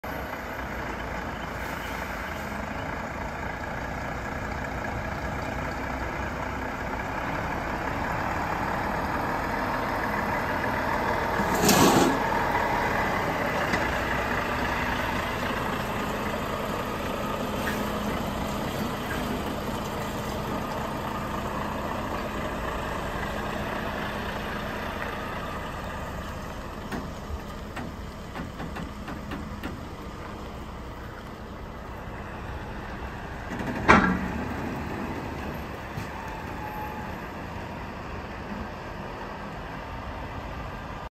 Big trucks.Monaghan Freights FH500 Volvo moving a wide load using our 4 axle Faymonville low loader.
Monaghan Freight moving a 4.20m wide load from Strabane, Northern Ireland to Sligo.